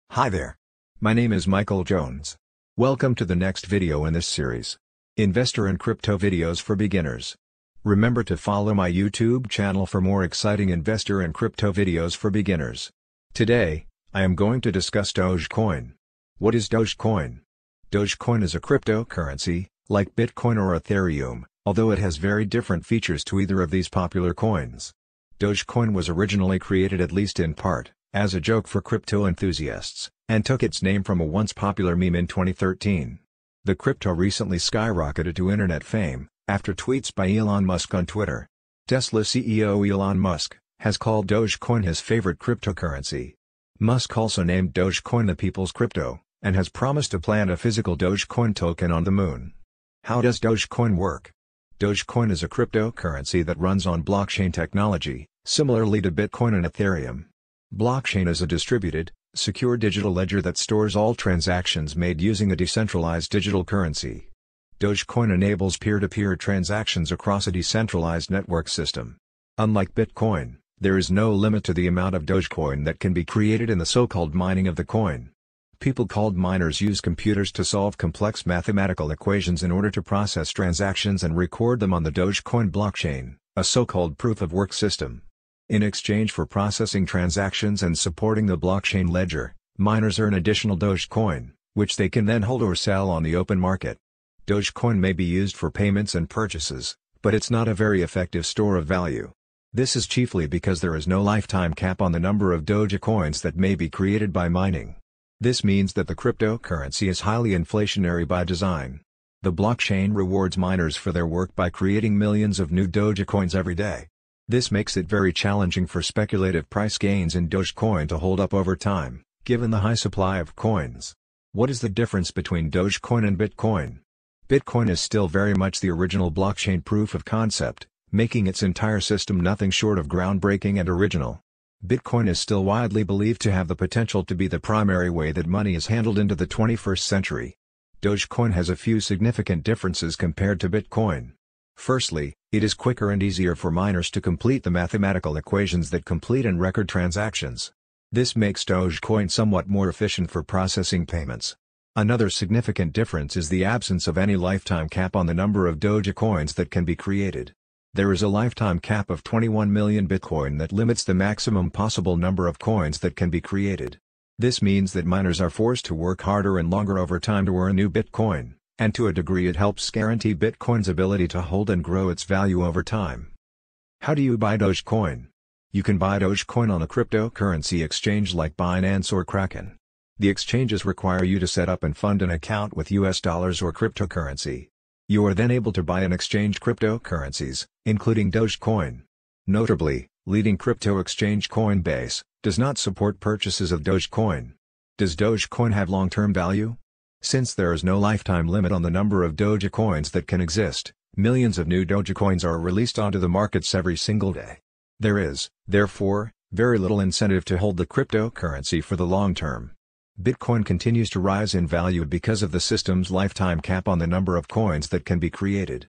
What is Dogecoin? How does Dogecoin work? What is the difference between Dogecoin and Bitcoin?
Hi there! My name is Michael Jones. Welcome to the next video in this series. Investor and Crypto videos for beginners. Today, I am going to discuss Dogecoin.
What is Dogecoin?
Dogecoin is a cryptocurrency, like Bitcoin or Ethereum—although it has very different features to either of these popular coins.
Dogecoin was originally created at least in part, as a joke for crypto enthusiasts, and took its name from a once-popular meme in 2013.  The crypto recently skyrocketed to internet fame, after tweets by Elon Musk on Twitter.
Tesla CEO Elon Musk, has called Dogecoin his favourite cryptocurrency. Musk also named Dogecoin the “people’s crypto,” and has promised to plant a physical Dogecoin token on the moon.
How does Dogecoin work?
Dogecoin is a cryptocurrency that runs on blockchain technology, similarly to Bitcoin and Ethereum. Blockchain is a distributed, secure digital ledger that stores all transactions made using a decentralized digital currency.
Dogecoin enables peer-to-peer transactions across a decentralized network system. Unlike Bitcoin, there is no limit to the amount of Dogecoin that can be created in the so-called mining of the coin.
People called miners use computers to solve complex mathematical equations in order to process transactions and record them on the Dogecoin blockchain—a so-called “proof of work” system. In exchange for processing transactions and supporting the blockchain ledger, miners earn additional Dogecoin, which they can then hold or sell on the open market.
Dogecoin may be used for payments and purchases, but it’s not a very effective store of value. This is chiefly because there is no lifetime cap on the number of Dogecoins that may be created by mining. This means that the cryptocurrency is highly inflationary by design. The blockchain rewards miners for their work by creating millions of new Dogecoins every day. This makes it very challenging for speculative price gains in Dogecoin to hold up over time, given the high supply of coins.
What is the difference between Dogecoin and Bitcoin?
Bitcoin is still very much the original blockchain proof-of-concept, making its entire system nothing short of ground-breaking and original. Bitcoin is still widely believed to have the potential to be the primary way that money is handled into the 21st century.
Dogecoin has a few significant differences compared to Bitcoin. Firstly, it is quicker and easier for miners to complete the mathematical equations that complete and record transactions. This makes Dogecoin somewhat more efficient for processing payments.
Another significant difference is the absence of any lifetime cap on the number of Dogecoins that can be created. There is a lifetime cap of 21 million Bitcoin that limits the maximum possible number of coins that can be created. This means that miners are forced to work harder and longer over time to earn new Bitcoin, and to a degree it helps guarantee Bitcoin’s ability to hold and grow its value over time.

How do you buy Dogecoin?
You can buy Dogecoin on a cryptocurrency exchange like Binance or Kraken. The exchanges require you to set up and fund an account with U.S. dollars or cryptocurrency. You are then able to buy and exchange cryptocurrencies, including Dogecoin. Notably, leading crypto exchange Coinbase, does not support purchases of Dogecoin.
Does Dogecoin have long-term value?
Since there is no lifetime limit on the number of Dogecoins that can exist, millions of new Dogecoins are released onto the markets every single day. There is, therefore, very little incentive to hold the cryptocurrency for the long term. Bitcoin continues to rise in value because of the system’s lifetime cap on the number of coins that can be created.